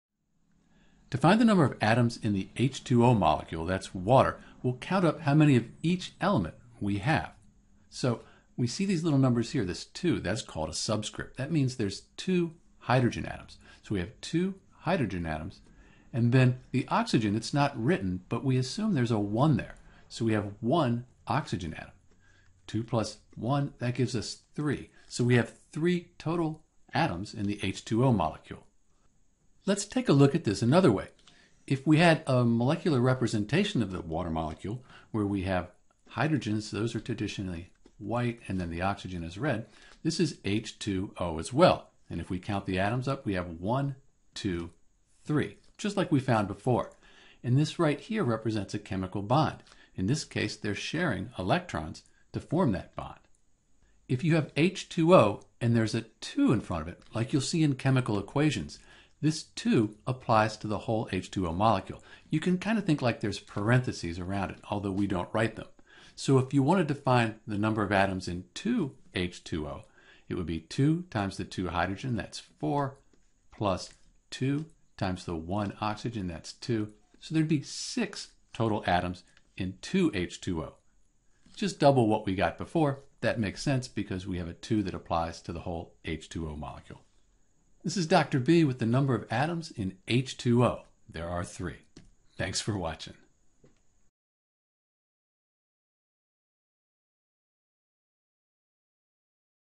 How to Find the Number of Atoms in H2O
To find the number of atoms in H2O (Water) we’ll add up the number of atoms for H and O.  The small number after the element symbol is called the subscript and this tells us how many of that atom are in the compound.  For example H2 means there are two Hydrogen atoms.

When there is no number after an element, as is the case with Oxygen (O), we assume there to be one atom of that element.  For example, in H2O we have only one O atom.

If there is a number in front of the compound, like 2H2O we multiple the subscripts for each element by the number in front (it’s called a “coefficient”).  For 2H2O we have four H atoms (the coefficient 2 times the subscript 2 and 2 O atoms).
For H2O there are a total of three atoms in the compound.